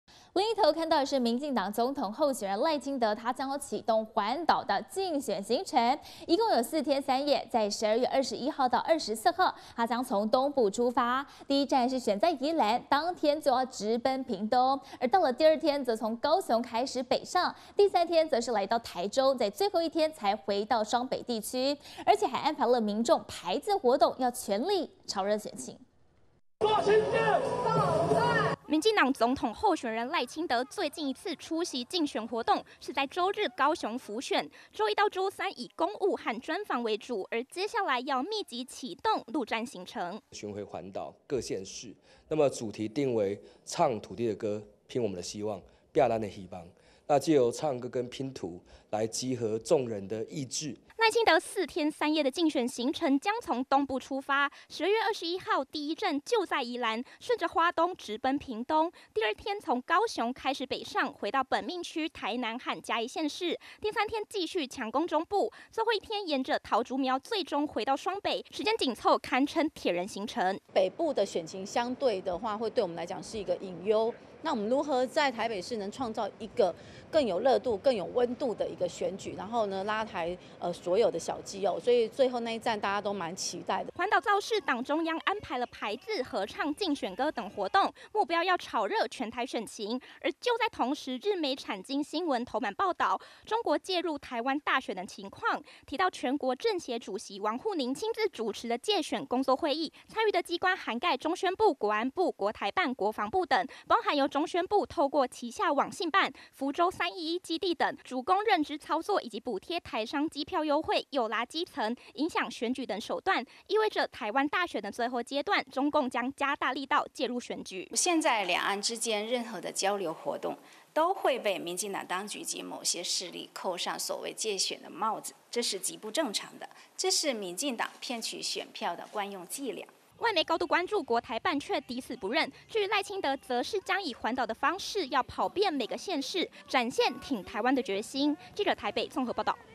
賴將啟動4天環島競選 首日從宜蘭到屏東
民進黨總統候選人賴清德將啟動環島競選行程，一共4天3夜，12月21日到24日，從東部出發，第一站選在宜蘭，當天就直奔屏東，第二天從高雄開始北上，第三天到台中，最後一天回到雙北，更安排了民眾排字活動，要全力炒熱選情。